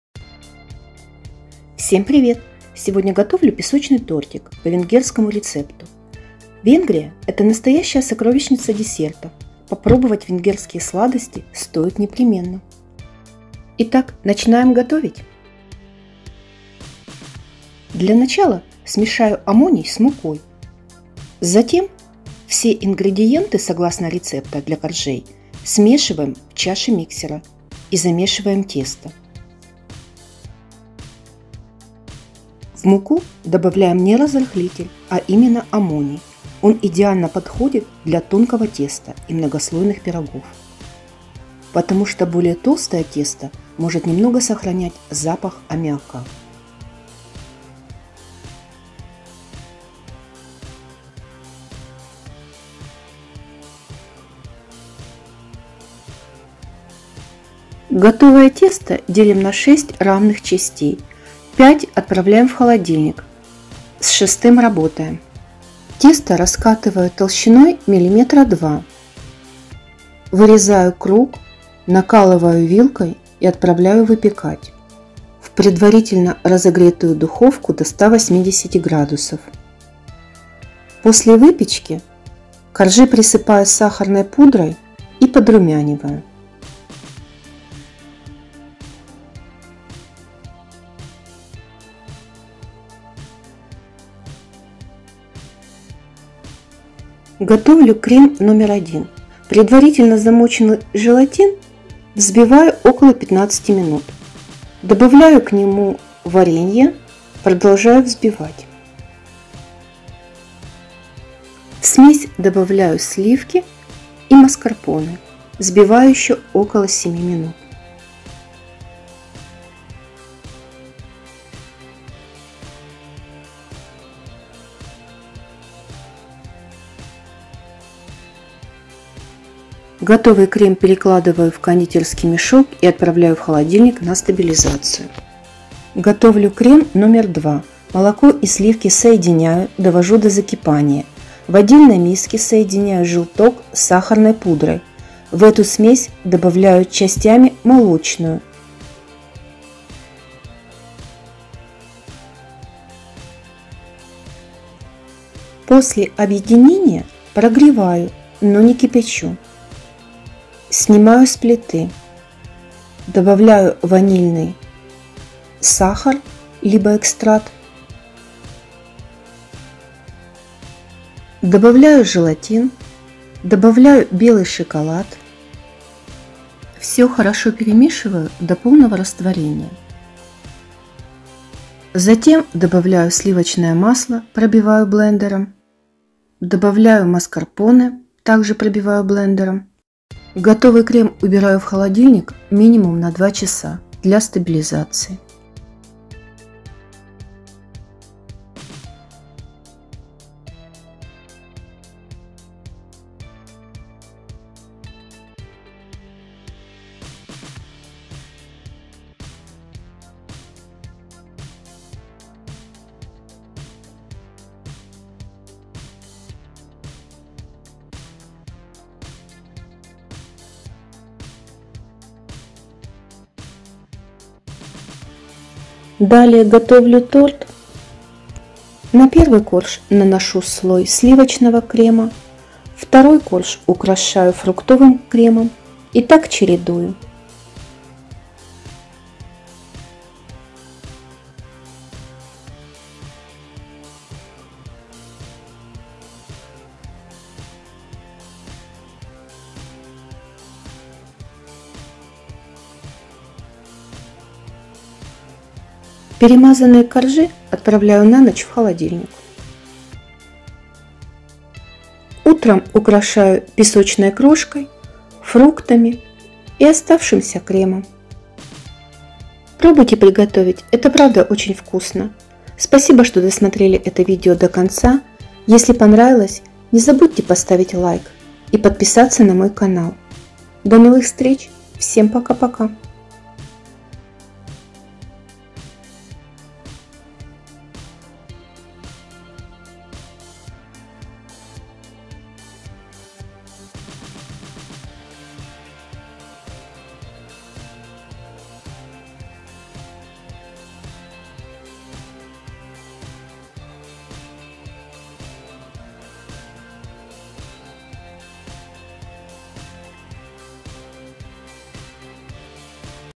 САМЫЙ ВКУСНЫЙ ТОРТ ИЗ ВЕНГРИИ- Hatlapos – шестислойный торт.
Hatlapos – это довольно известный в Венгрии домашний торт. В переводе с венгерского его название означает «шестислойный».
Песочный торт.
Рецепт:
Для теста:
Жир утиный 60 гр
Сахарная пудра  150 гр
Ванилин
Сметана 35 гр
Белок яиц 3 шт
Мёд 1 столовая ложка
Амоний 8 гр
Для крема 1:
Варенье 200 гр
Сливки 200 гр
Желатин 7 гр +вода 50 гр
Маскарпоне 150 гр

Для крема 2:
Сливки 33% 175 гр
Молоко 150 гр
Желток яйца 3 шт
Сахар 50 гр
Желатин 5 гр
Шоколад белый 100 гр
Масло сливочное 100 гр
Маскарпоне 200 гр